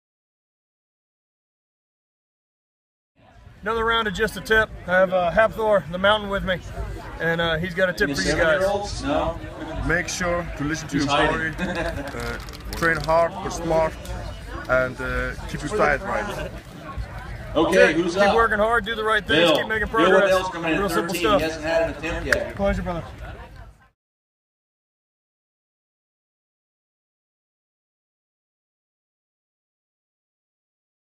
Just the Tip: Advice from the Mountain.
Thor-

The mountain lets everyone know you have to cover it from all sides.  Training hard, nutrition, and recovery.  Listening to your body is the best thing an athlete can learn to do.

Know when to push and when to back off and Deload.